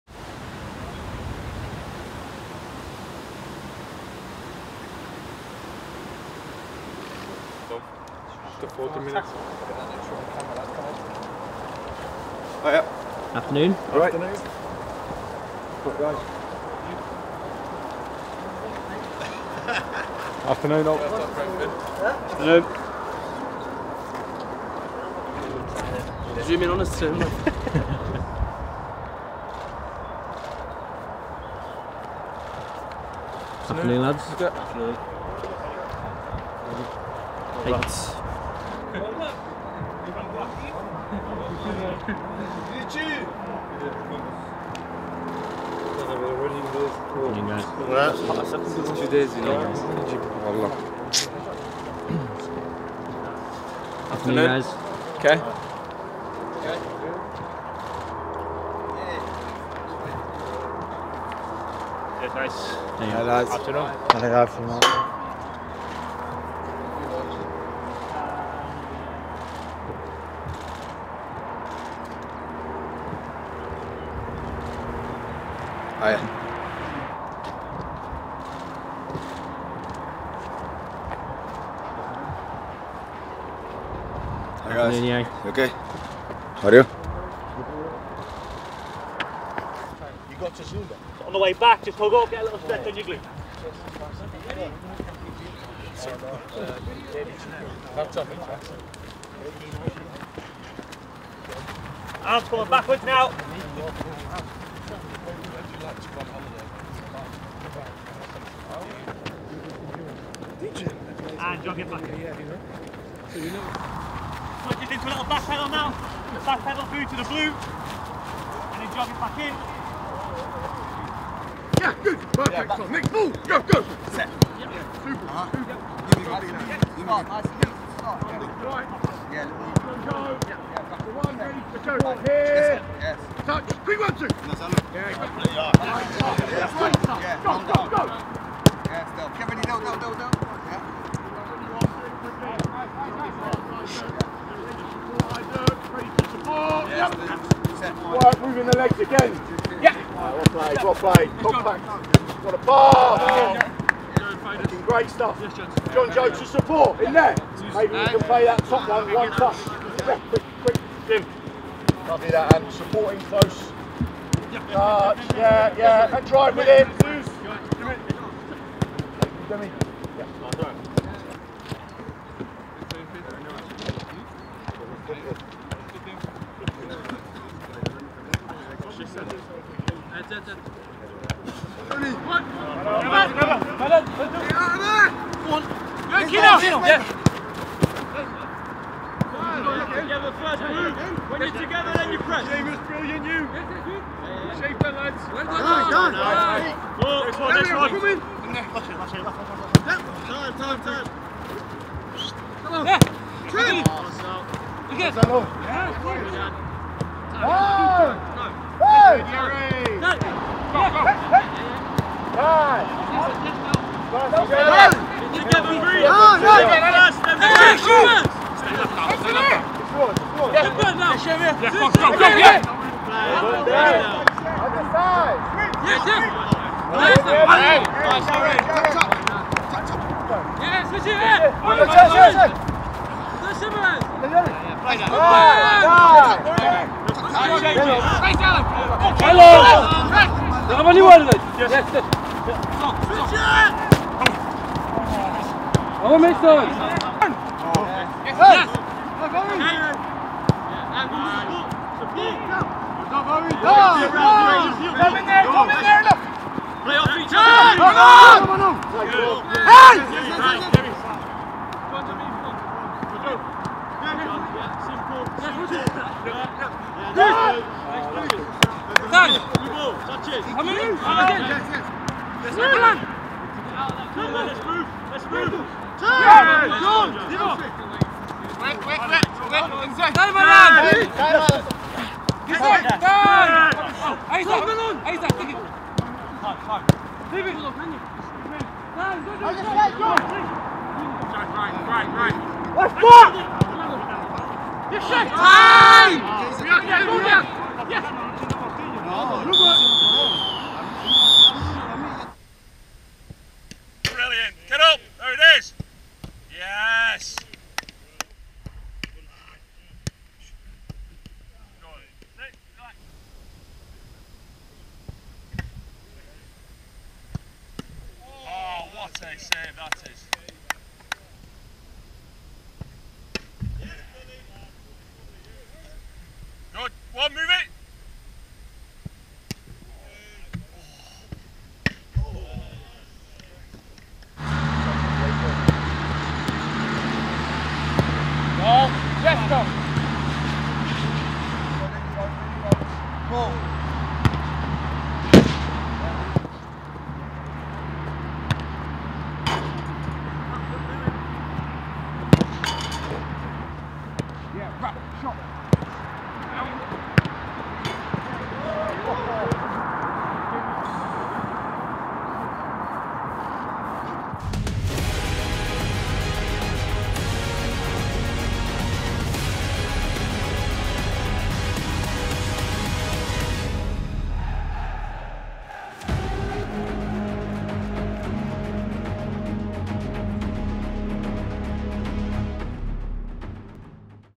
TOM DAVIES RETURNS TO TRAINING AS BLUES PREPARE FOR CHELSEA
Midfielder Tom Davies returned to training at Finch Farm this week after missing most of the season via a hamstring injury and subsequent surgery. Meanwhile, the rest of the Blues squad focussed on the next task at hand - Chelsea at home on Sunday…